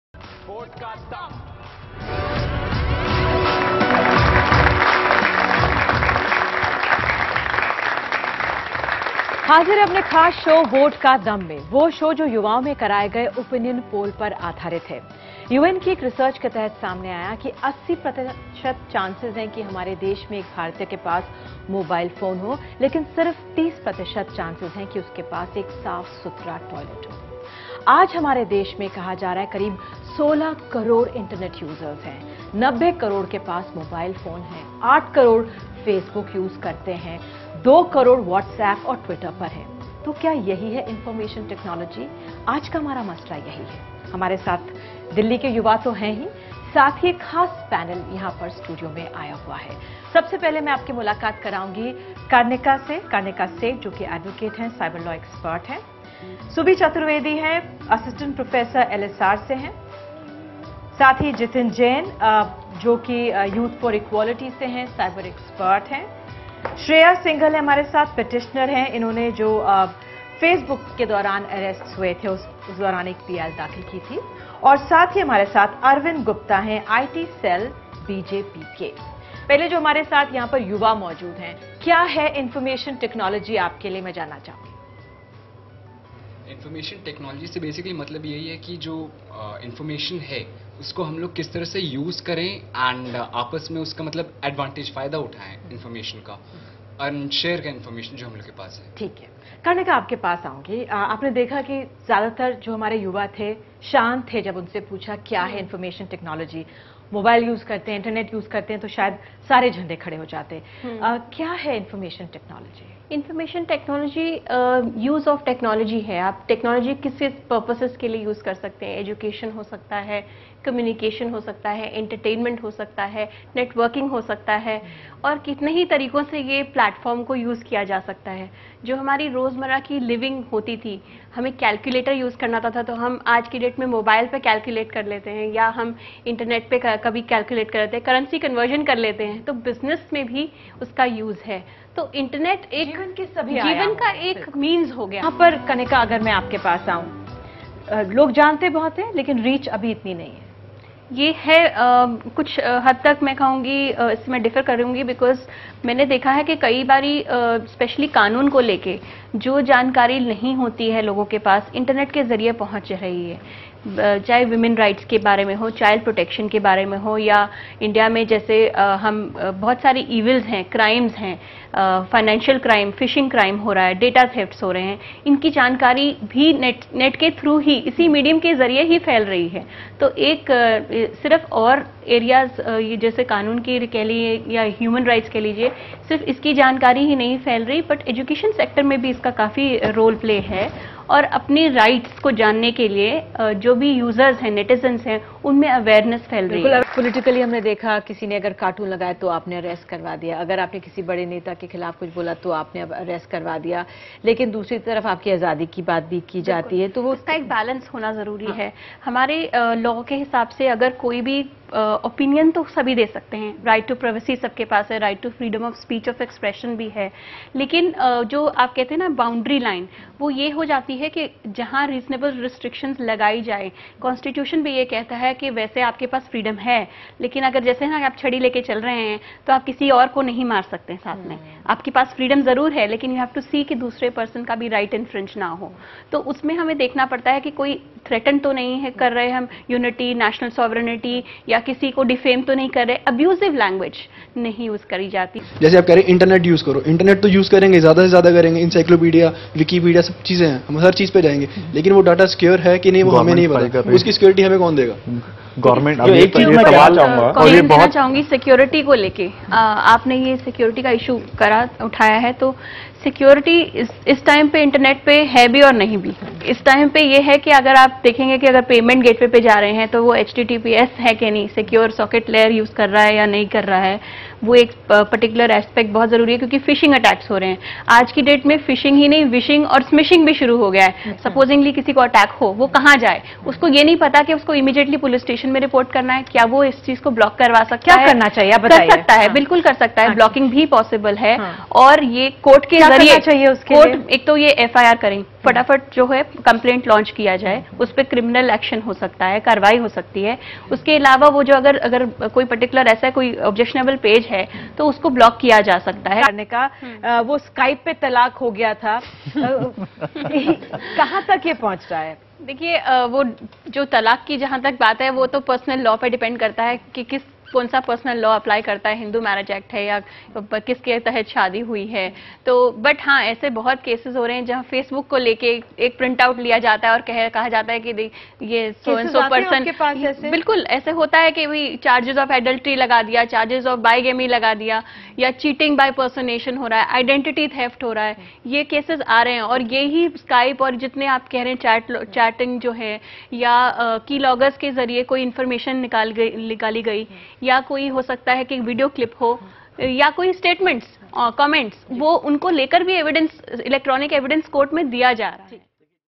Information Technology and Young India ( edited version)
The discussion was on the IT Act, its basic provisions, what changes can be made in the Act. Karnika Seth, Cyber Law Expert was invited to share information on the Information Technology Act. Karnika Seth discussed the various uses of the Technology like chatting, online shopping; people use calculators on their phones, currency conversion. Internet has become a convergence of digital technologies. Internet is widely accessed and people use internet to get information on something related to law, education, women rights, rights of children etc. Use of internet is there in all the sectors. She also added that netizens must also be aware of their responsibility and duties along with their rights. With freedom, there comes a responsibility and duty towards others. The integrity, public morality, decency sovereignty of India must be respected. She also discussed the issues related to cyber security and different cyber crimes emerging like phishing attacks, smishing attacks and these crimes must be reported to the Police so that an FIR can be registered and immediate action can be taken.

Connect With Karnika Seth at